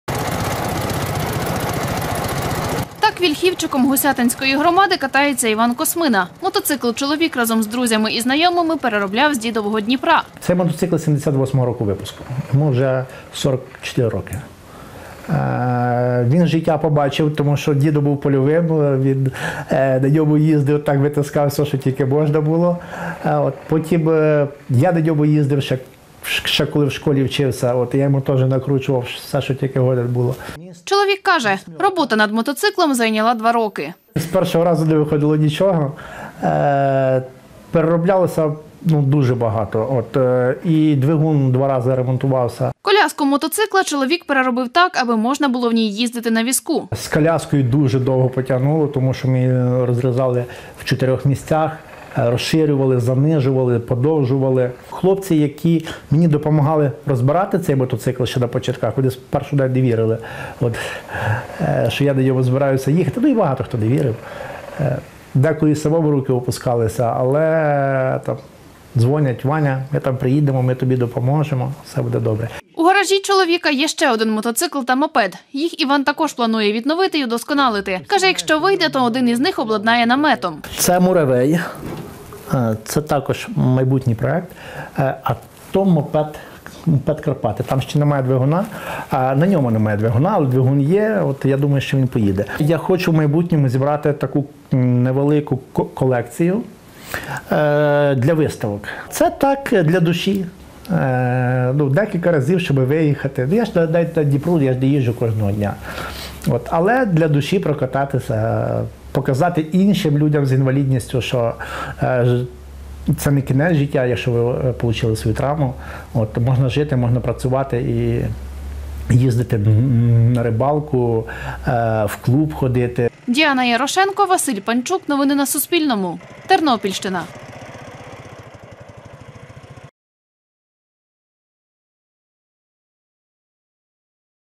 На Тернопільщині чоловік з інвалідністю відновлює мотоцикли своєї родини
Відновити усі мотоцикли своєї родини хоче Іван Космина з села Вільхівчик Гусятинської громади. Чоловік пересувається на візку, тож транспортні засоби адаптовує під себе. Він розповів, коли працював над першим мотоциклом – сусіди не вірили, що він поїде.